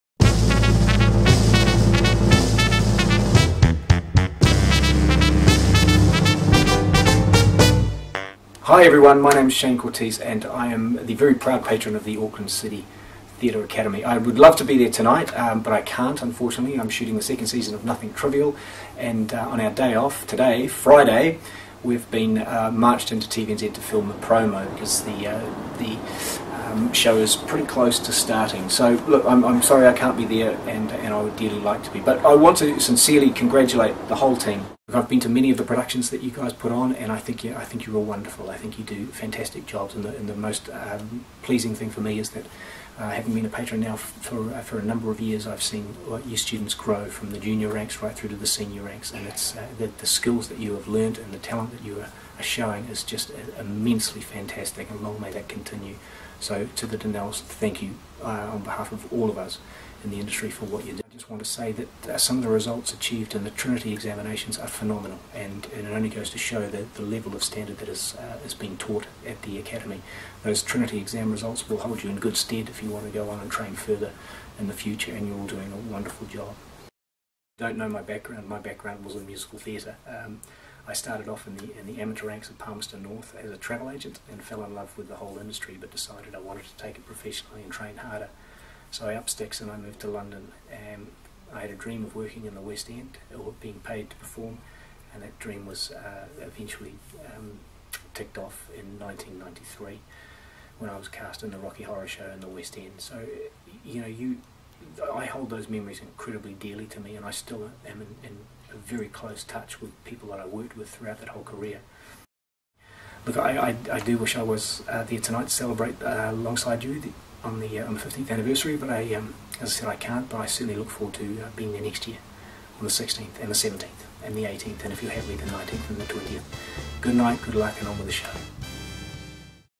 ACTA's 15th Anniversary Showcase Video Message from Shane Cortese (Patron)
Check out the special video message from Auckland City Theatre Academy's Patron Shane Cortese for our 15th Anniversary Showcase.